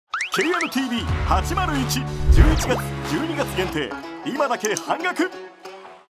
【KNTV801】2021年11月・12月限定半額キャンペーン！
新規契約者全員半額！
韓流エンタメを最大2カ月見放題✨